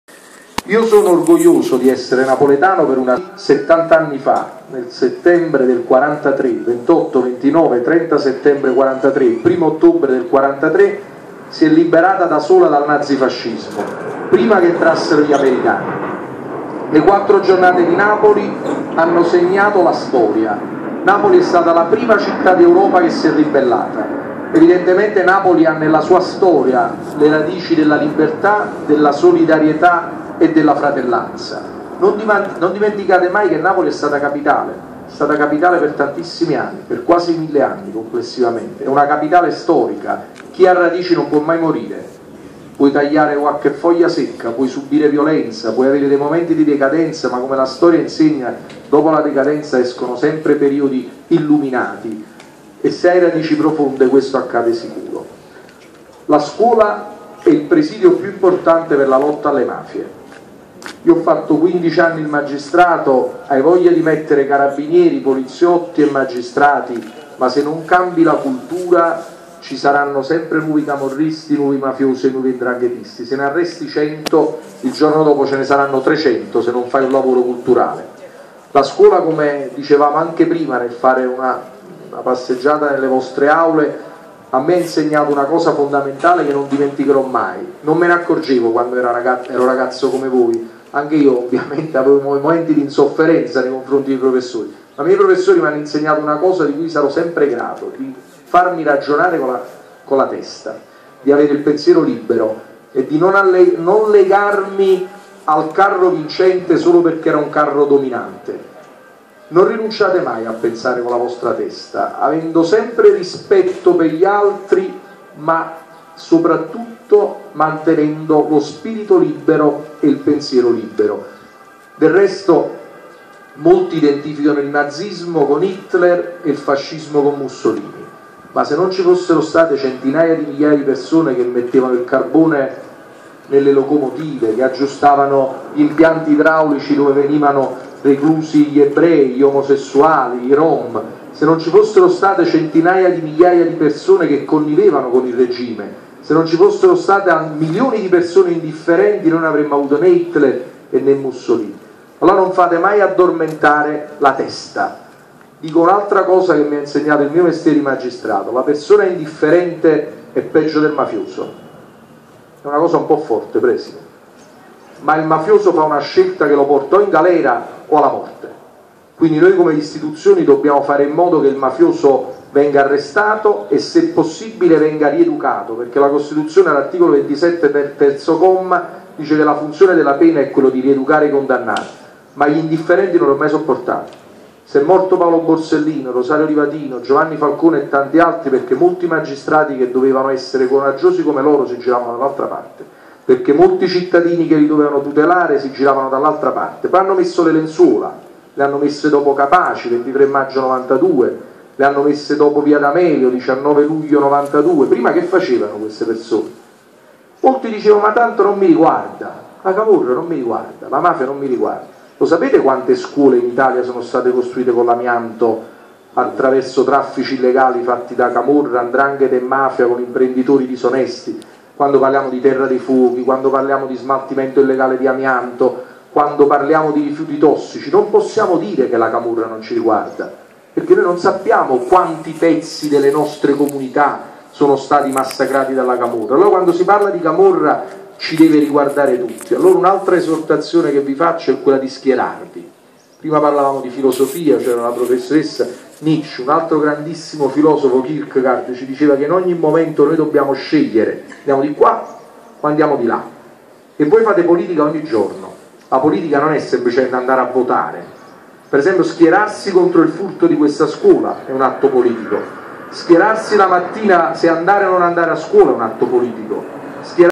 Discorso sulla liberazione di Luigi De Magistris (Liceo Scientifico Francesco Sbordone)
"L'indifferente è peggio del mafioso".